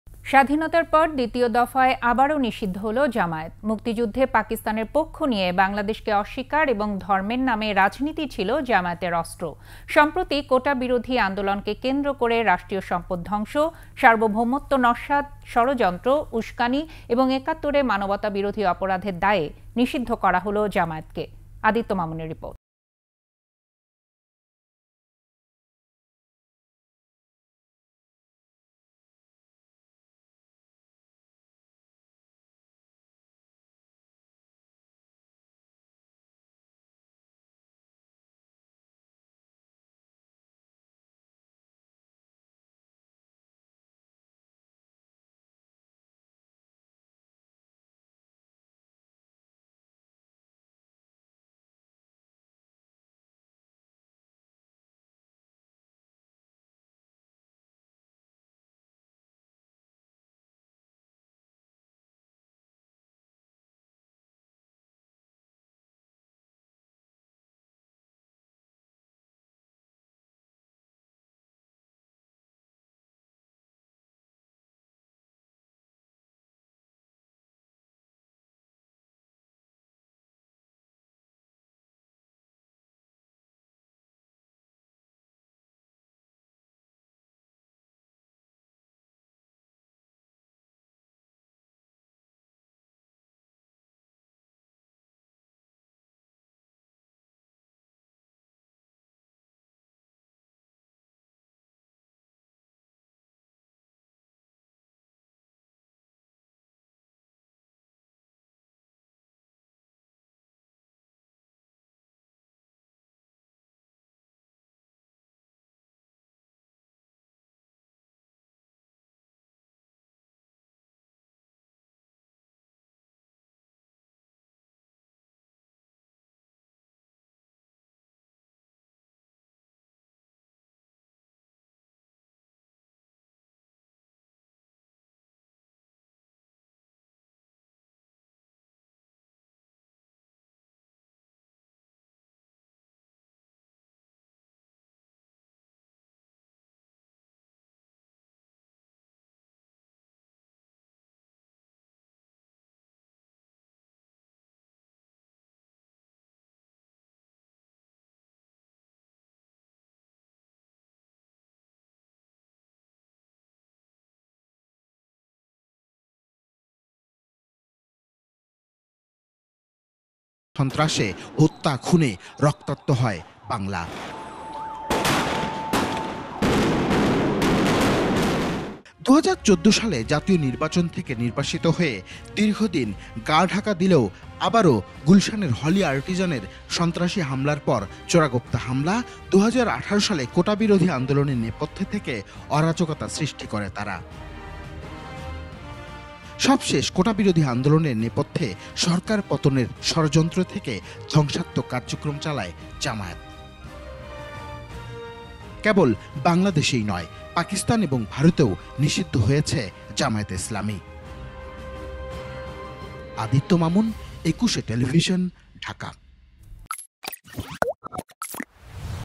দ্বিতীয় দফায় আবারও নিষিদ্ধ হলো জামায়াত | ETV News
স্বাধীনতার পর দ্বিতীয় দফায় আবারও নিষিদ্ধ হলো জামায়াত। মুক্তিযুদ্ধে পাকিস্তানের পক্ষ নিয়ে, বাংলাদেশকে অস্বীকার এবং ধর্মের নামে রাজনীতিই ছিল জামায়াতের অস্ত্র। সম্প্রতি কোটাবিরোধী আন্দোলনকে কেন্দ্র করে রাষ্ট্রীয় সম্পদ ধ্বংস, সার্বভৌমত্ব নস্যাৎ, ষড়যন্ত্র, উস্কানি এবং একাত্তরে মানবতাবিরোধী অপরাধের দায়ে নিষিদ্ধ করা হলো জামায়াতকে। আদিত্য মামুনের রিপোর্ট।